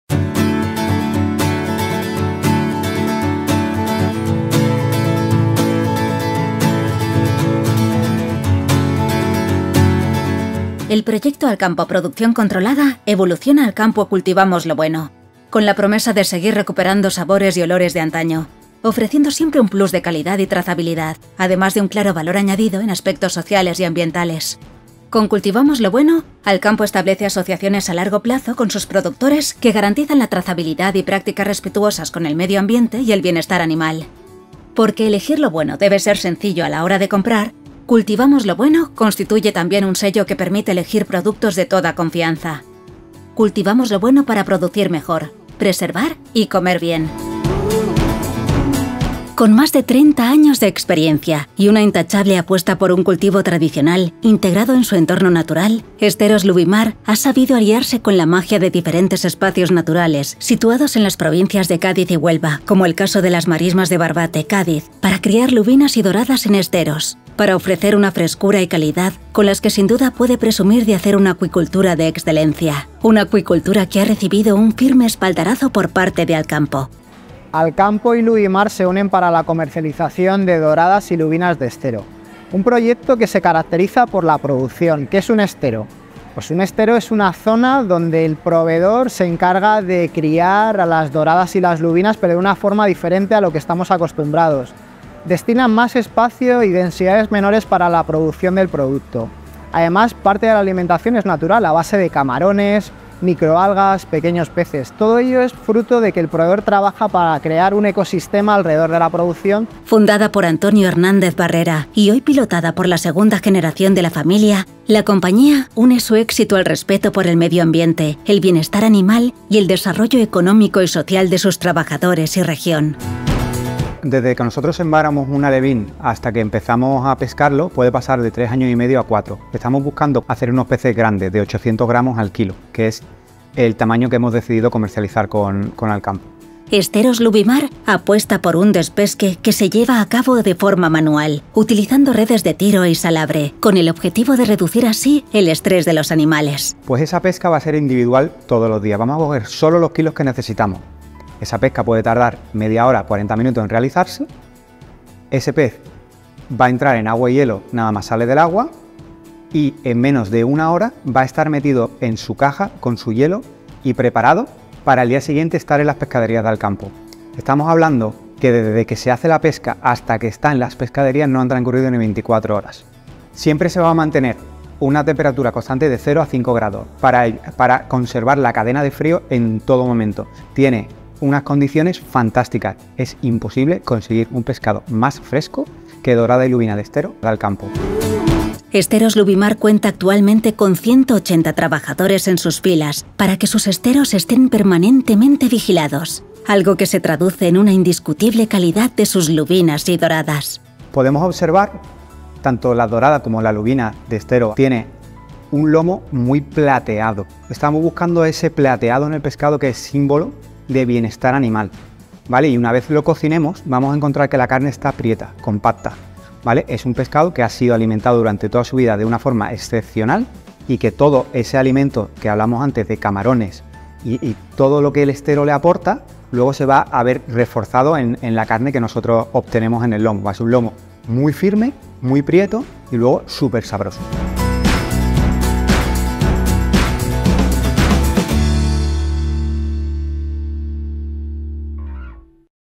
Alcampo | Cultivamos Lo Bueno: Esteros Lubimar
Descubre el sabor único de la lubina Estero Lubimar.

Nuestra lubina garantiza frescura y calidad superior. ¡Prueba lo mejor del mar!

¡Síguenos y no te pierdas nada!